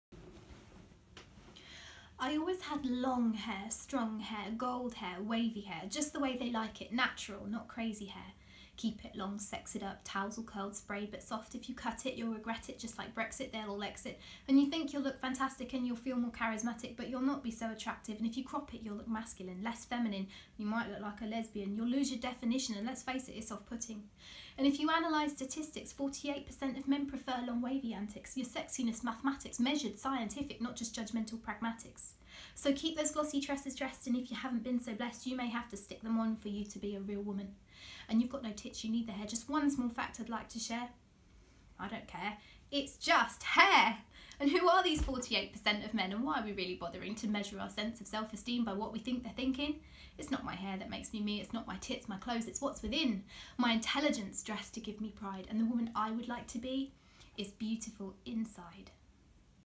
Rebel Hair?
Should we go for the big chop or keep it long